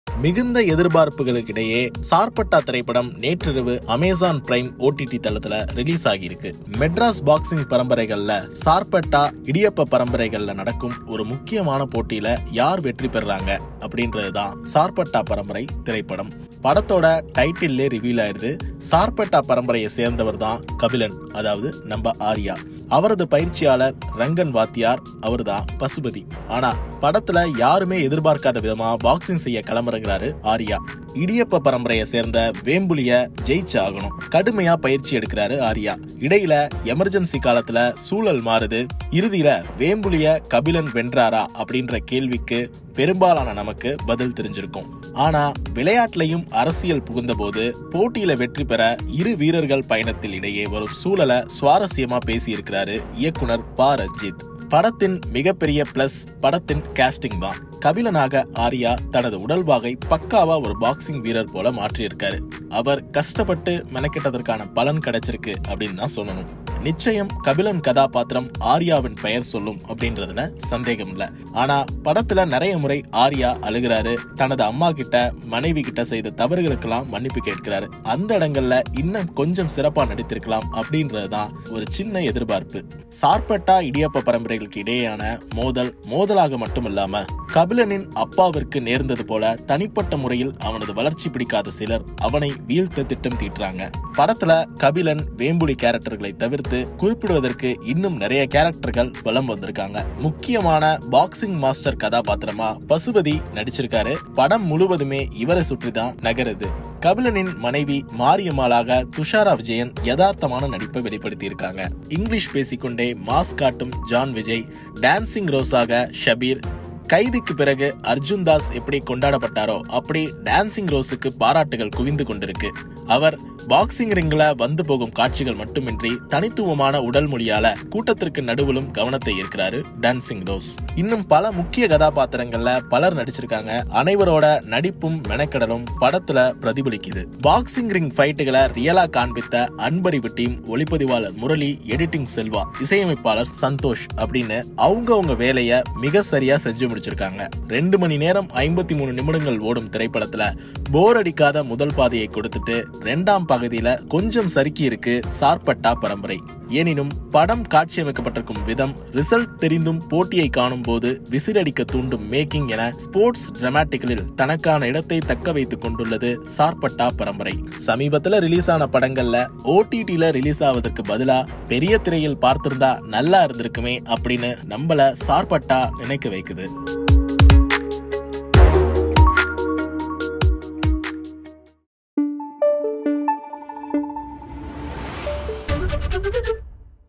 சார்பட்டா பரம்பரை - இது பா.ரஞ்சித்தின் நாக் அவுட் பஞ்ச் | Sarpatta parambarai movie review | Ranjith
மெட்ராஸ் பாக்ஸிங் பரம்பரைகளில் சார்பட்டா- இடியப்ப பரம்பரைகளில் நடக்கும் ஒரு முக்கியமான போட்டியில் யார் வெற்றி பெறுவார் என்பதுதான் சார்பட்டா பரம்பரை திரைப்படம்.

படத்தின் டைட்டிலே சொல்லிவிடுவதால், இதில் சார்பட்டா பரம்பரையைச் சேர்ந்தவர்தான் கபிலன் (ஆர்யா), அவரது பயிற்சியாளர் ரங்கன் வாத்தியார் (பசுபதி). ஆனால், யாரும் எதிர்பாராத விதமாக பாக்ஸிங் செய்ய களமிறங்குகிறார் ஆர்யா. இடியப்ப பரம்பரையைச் சேந்த வேம்புலியை (ஜான் கொக்கேன்) வென்றாக வேண்டும். பயிற்சி எடுக்கிறார் ஆர்யா, இடையில் எமெர்ஜென்ஸி காலத்தால் சூழல் மாறுகின்றது. இறுதியில், வேம்புலியை கபிலன் வென்றான என்றால், பெரும்பாலும் அனைவருக்கும் இதற்கான பதில் தெரிந்திருக்கும். ஆனால், விளையாட்டினும் அரசயில் புகுந்தபோதும், போட்டியில் வெற்றி பெறுவதற்கான இரு வீரர்களின் பயணத்தில் இடையே வரும் சூழலை சுவாரஸ்யமாக பேசியிருக்கிறார் இயக்குநர் பா.ரஞ்சித்.

படத்தின் மிகப்பெரிய ப்ளஸ், படத்தின் கேஸ்டிங். கபிலனாக ஆர்யா. தனது உடல்வாகை பக்காவாக ஒரு பாக்ஸிங் வீரர் போல மாற்றியமைக்க மெனகெட்டதற்கு பலன் கிடைத்துள்ளது. கபிலன் கதாப்பாத்திரம் ஆர்யாவின் பெயர் சொல்லும். படத்தில் நிறைய முறை அழுகிறார், தனது அம்மாவிடம் மனைவியிடம் செய்த தவறுகளுக்காக மன்னிப்பு கேட்கிறார், அந்த இடங்களில் இன்னும் கொஞ்சம் சிறப்பாக நடித்திருக்கலாம்.

சார்பட்டா – இடியப்ப பரம்பரைகளுக்கு இடையேயான மோதமாக மட்டுமில்லாமல், கபிலனின் அப்பாவிற்கு நேர்ந்தது போல, தனிப்பட்ட முறையில் அவனது வளர்ச்சி பிடிக்காத சிலர் அவனை வீழ்த்த திட்டம் தீட்டுகிறார்கள்.

படத்தில், கபிலன், வேம்புலி கேரக்டர்களை தவிர்த்து குறிப்பிடுவதற்கு இன்னும் நிறைய கேரக்டர்கள் இருக்கின்றன. முக்கியமான கதாப்பாத்திரமாக பாக்ஸிங் மாஸ்டராக பசுபதி, படம் என்னவோ இவரைச் சுற்றிதான் நகரும். கபிலனின் மனைவி மாரியம்மாளாக துஷாரா விஜயன், யதார்த்தமாக நடித்துள்ளார். டேடி கதாப்பாத்திரத்தில் இங்கிலிஷ் பேசிக்கொண்டே மாஸ் காட்டும் ஜான் விஜய், டேன்சிங் ரோசாக ஷபீர். கைதிக்கு பிறகு அர்ஜூன் தாஸ் எப்படி கொண்டாடப்பட்டாரோ அப்படி டேன்சிங் ரோஸிற்கு பாராட்டுக்கள் குவிந்து கொண்டிருக்கின்றன. அவர் பாக்ஸிங் ரிங்கில் வந்துபோகும் காட்சிகள் மட்டுமின்றி, தனித்துமான உடல் மொழியால் கூட்டத்தின் நடுவிலும் கவனத்தை ஈர்க்கிறார். இன்னும் பல முக்கிய கதாப்பாத்திரங்களில் பலர் நடித்துள்ளனர். அனைவரது நடிப்பும், மெனக்கெடலும் படத்தில் தெரிகிறது, அந்தந்த கதாப்பாத்திரங்களாகவே வந்து போகின்றனர்.

பாக்ஸிங் ரிங் ஃபைட்டுகளை ரியலாக காண்பித்த அன்பறிவு டீம், ஒளிப்பதிவாளர் முரளி, எடிட்டிங் செல்வா, இசையமைப்பாளர் சந்தோஷ் என அவரவர் பங்கினை சரியாக செய்து முடித்துள்ளனர்.

2 மணி நேரம் 53 நிமிடங்கள் ஓடும் திரைப்படத்தில், போர் அடிக்காத முதல் பாதியை கொடுத்துவிட்டு, இரண்டாம் பாதியில் சற்று சறுக்குகிறது சார்பட்டா பரம்பரை. எனினும், படம் காட்சியமைக்கப்பட்டிருக்கும் விதம், ரிசல்ட் தெரிந்தும் போட்டியை காணும்போது விசில் அடிக்க தூண்டும் மேக்கிங் என ஸ்போர்ட்ஸ் டிராமாக்களில் தனக்கான இடத்தை தக்க வைத்து கொண்டுள்ளது சார்பட்டா பரம்பரை! சமீபத்தில் ரிலீஸான படங்களில், ஓடிடியில் ரிலீஸாவதற்கு பதிலாக பெரிய திரையில் பார்த்திருக்கலாம் என நினைக்க வைத்தது சார்பட்டா!

CREDITS:
Script: Karthiga rajendiran
Editing: Vivekanandan
Voice Over: Arunmozhivarman